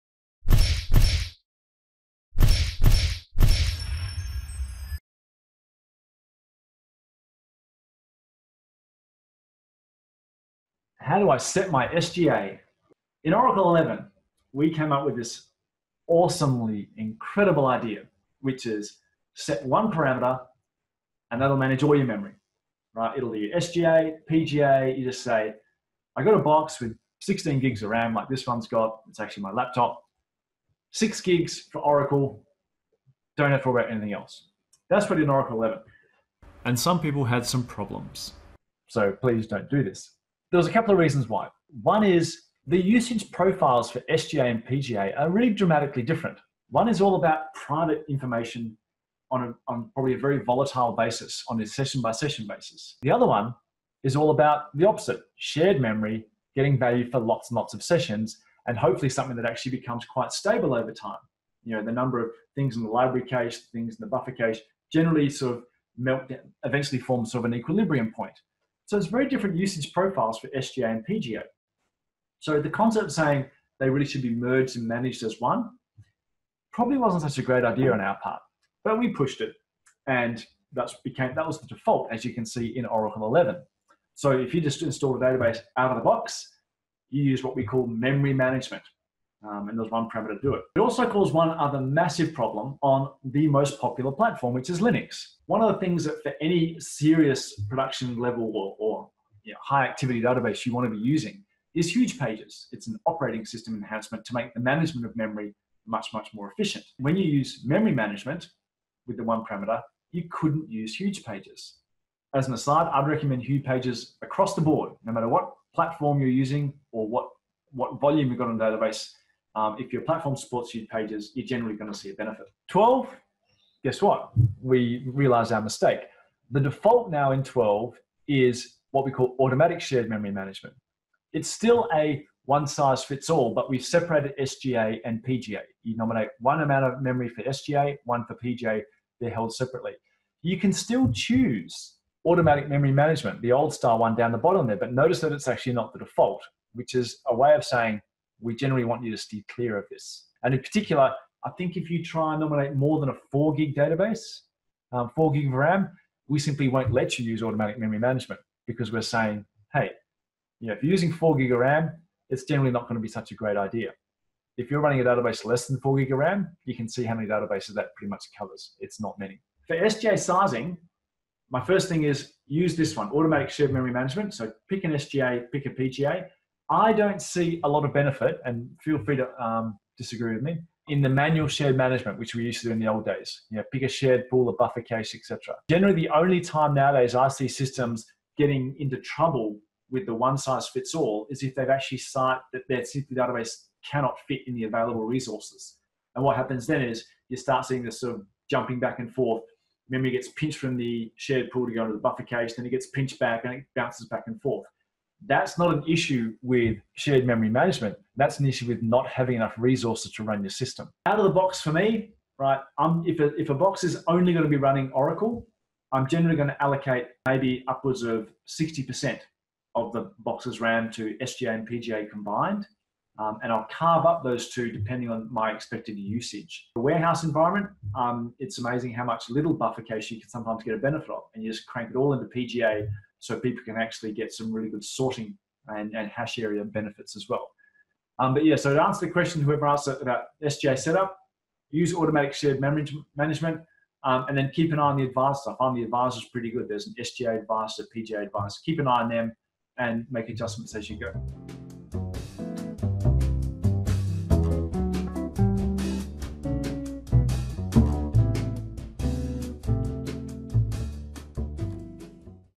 SGA, PGA, help me with all these GA's
A look at the memory structures used by the Oracle Database and how to size them appropriately when creating the database.

Are you serious? A free Oracle database forever ?! Oh yes indeed!